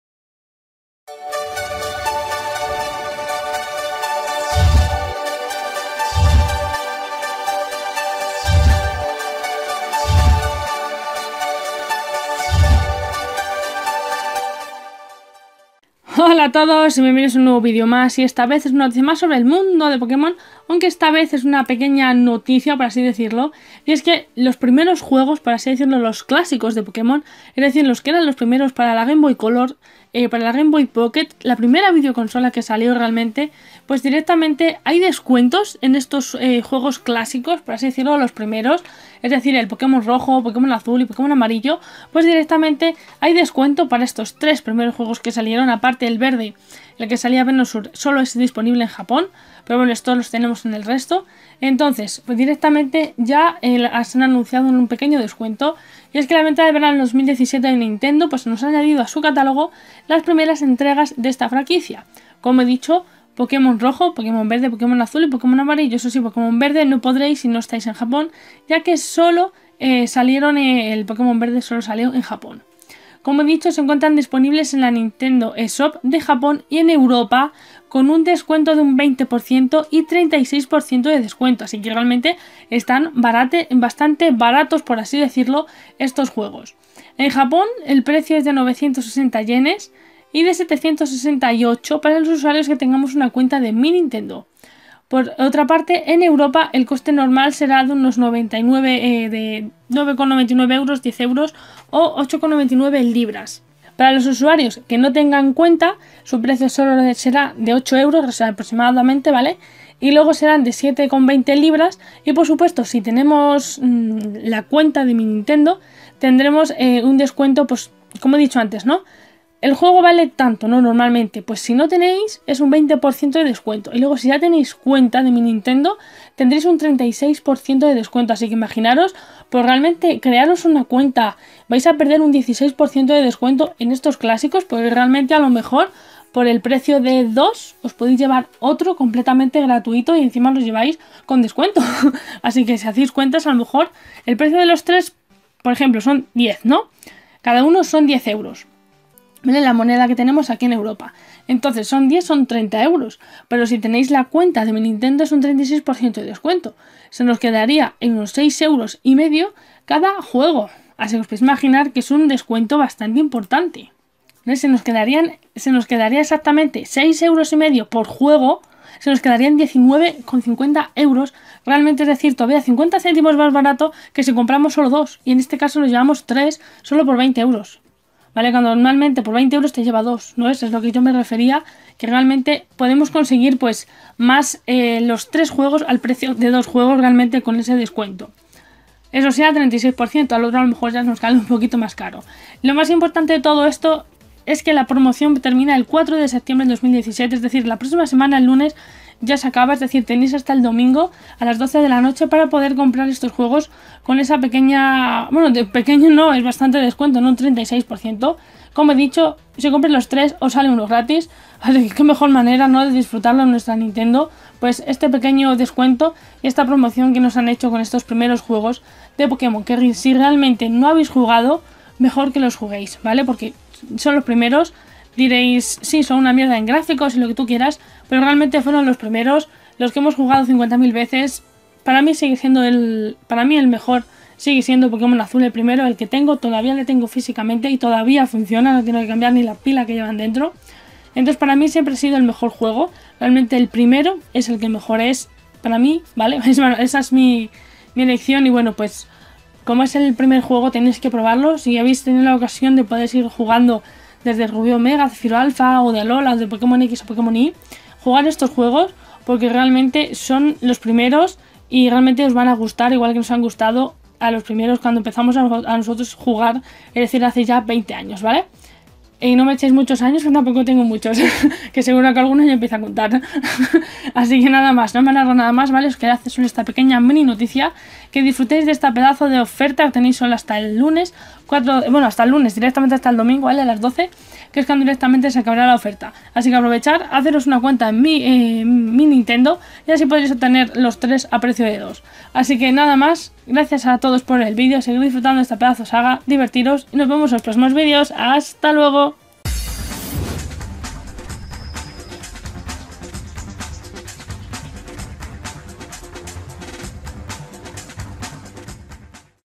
Descuento en los clásicos de Pokémon en la Nintendo eShop
Os cuento un poquito sobre este gran descuento en los clásicos de Pokémon, es la mejor oportunidad para disfrutar de ellos.